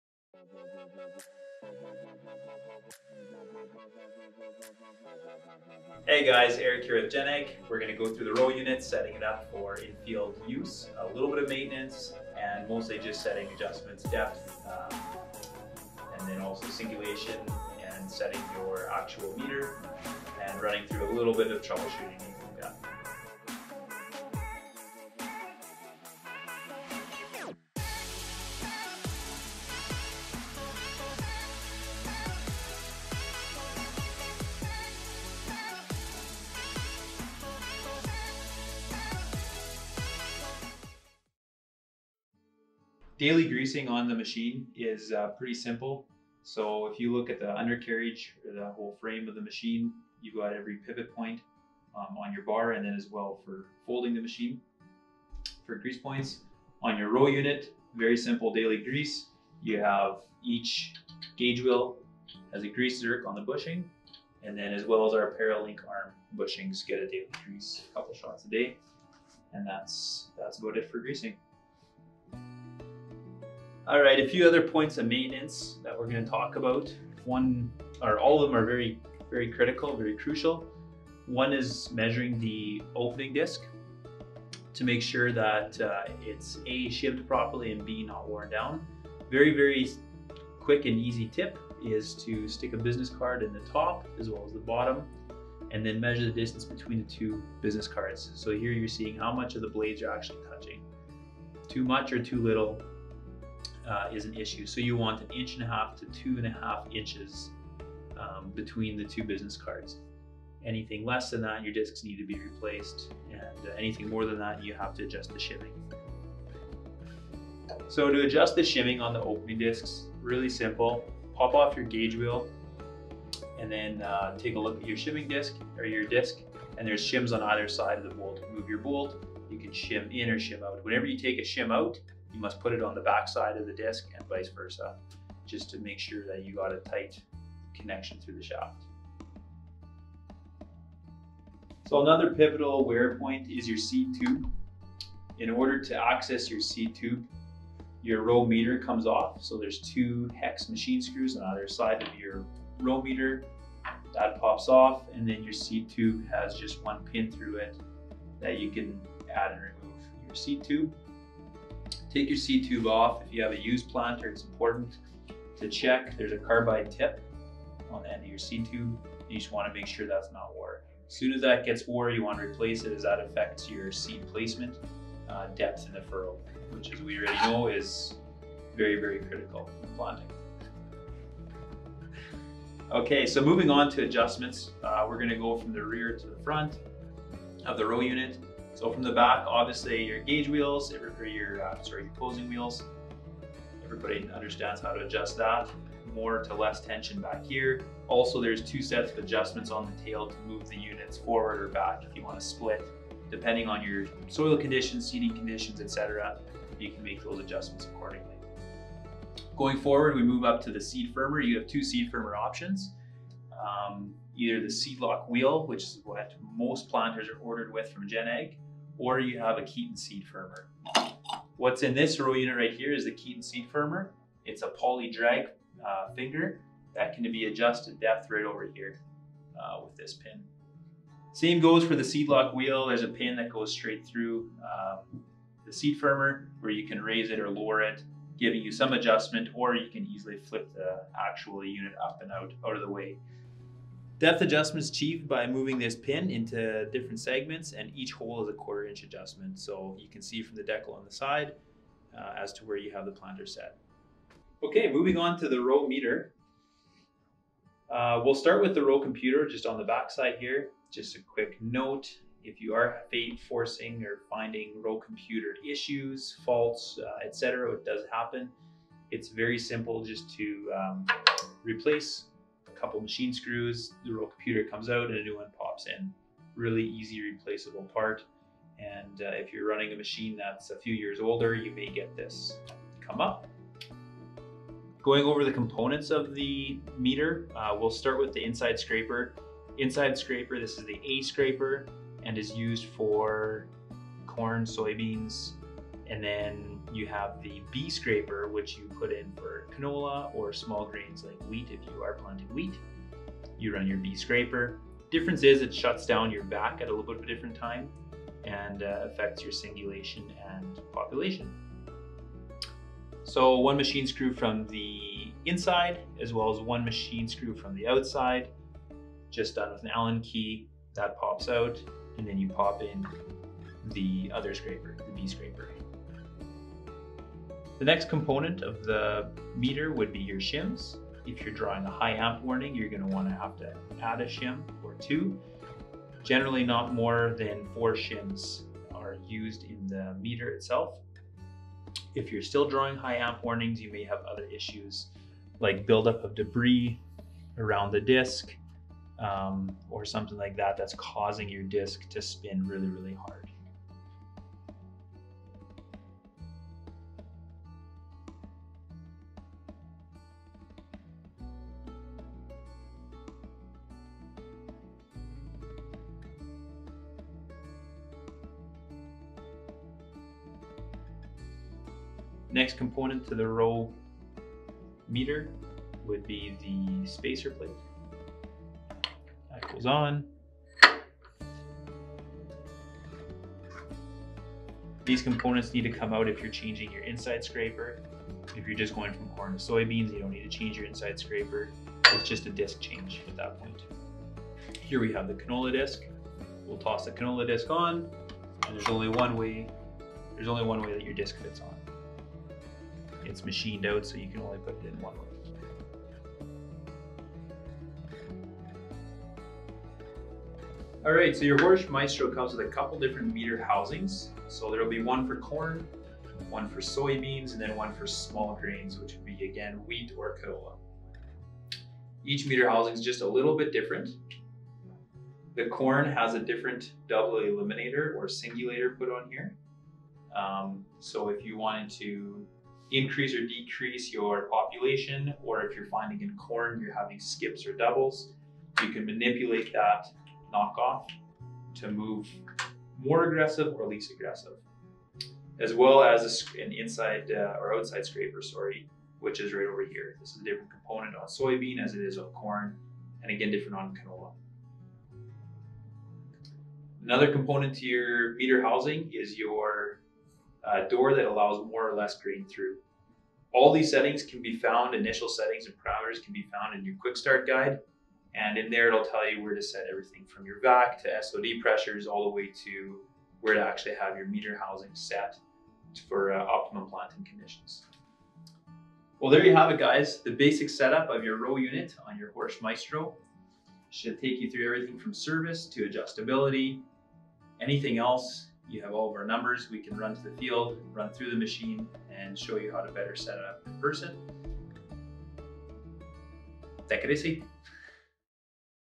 HORSCH Maestro Row Unit - Maintenance and Setup
HORSCH Maestro Row Unit Setup and Maintenance by Eric Braun of GenAG. If you require any further assistance with your Maestro contact GenAG and we would be happy to walk you through it, or make sure to check out the HORSCH Maestro 800 Terminal at the link below.